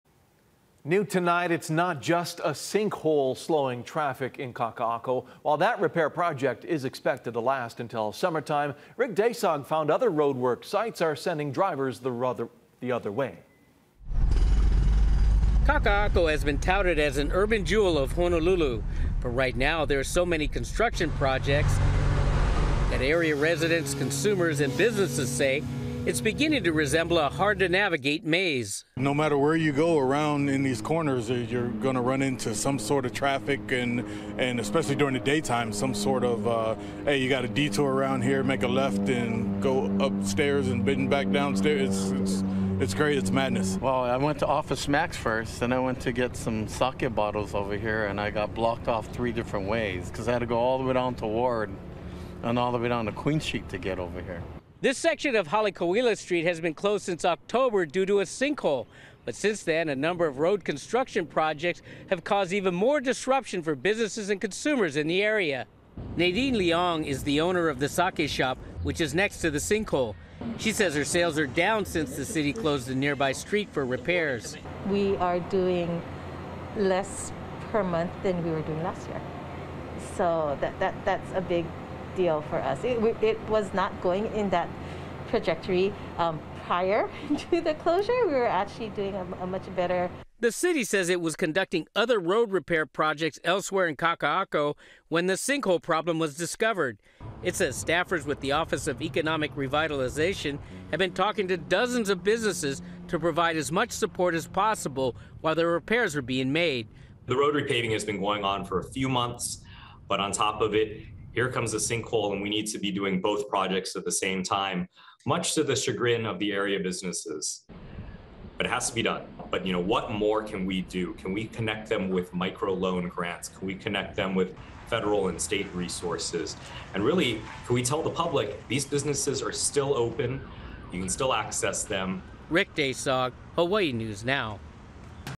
Kakaako shoppers, businesses grapple with traffic ‘madness’ as road repairs continue
“No matter where you go ... you’re gonna run into some sort of traffic."